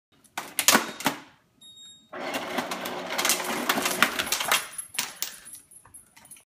Weimaraner Puppy Helping herself to some ice
7 Month old Lizzy taught herself how to dispense her own ice😂😂